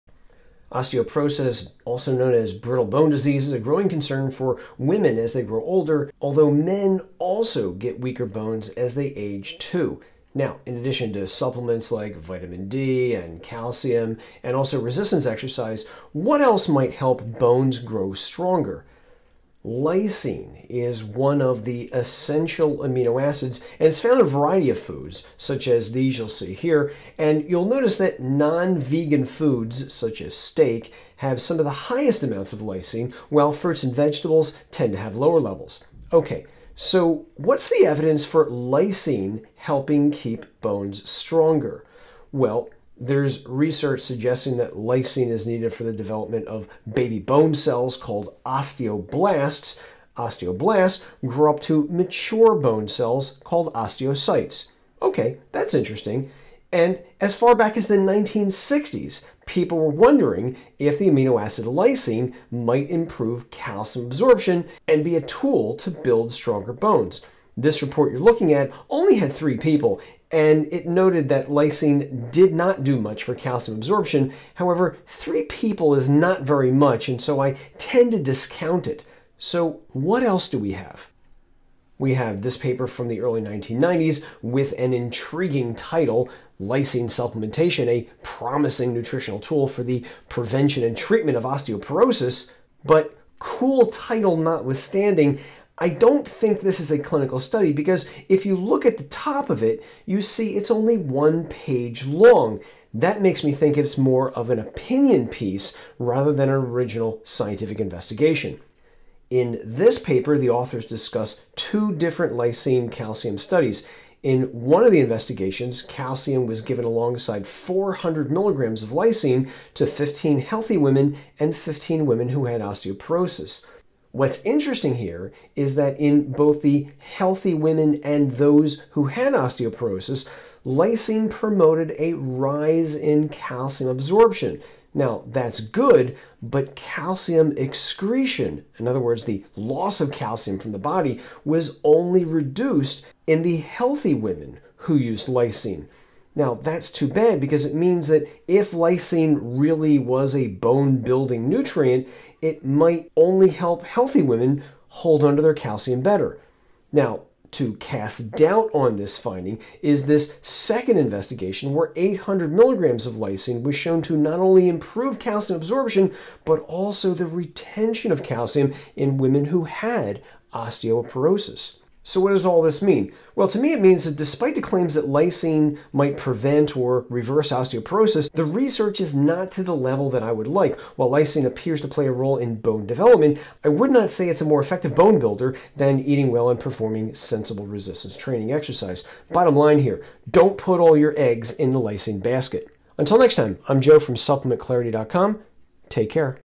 Is Lysine The Missing Osteoporosis Prevention Nutrient?
Can the amino acid lysine build bone and reverse osteoporosis? Here are the results from the clinical studies.
🔴 Here's a quality brand
🔴 🔴Things I like & use:
🔴🔴 🔴Things used to make this video:
1 Lot’s of work
2 This video editing software
3 This Microphone